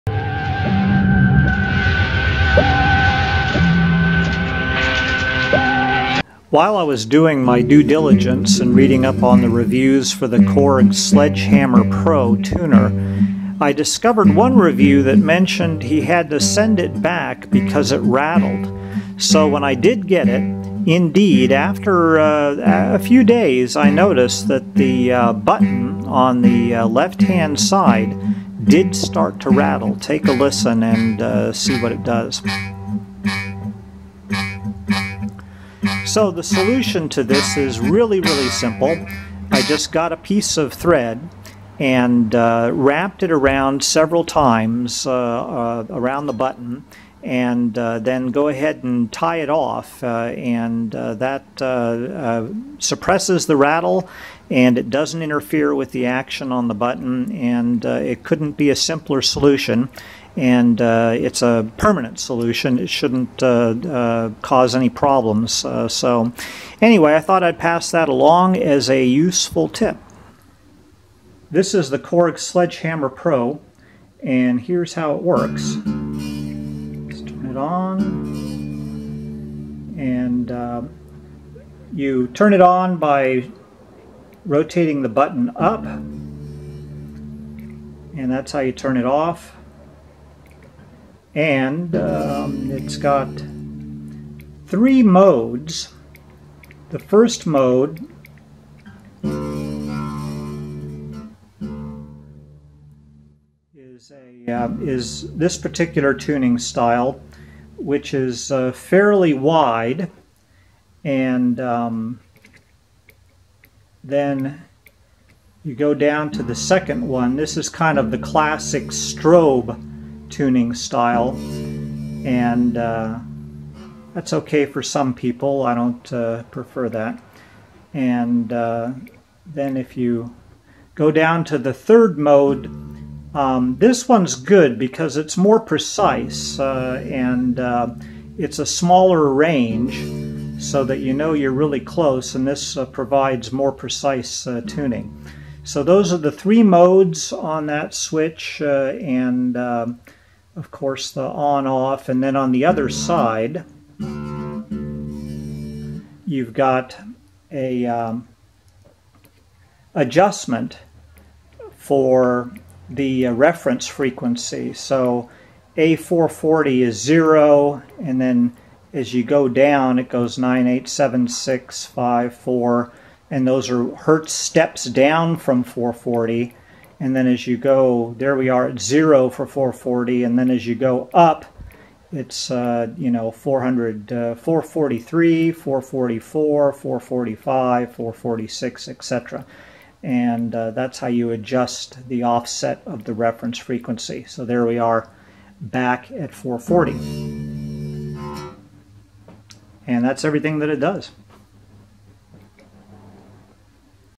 KORG TUNER - SLEDGEHAMMER PRO, RATTLE FIX & REVIEW!
SEE MY SIMPLE FIX FOR THE RATTLE PROBLEM AND A REVIEW OF THE FUNTIONS ON THIS QUITE GOOD TUNER.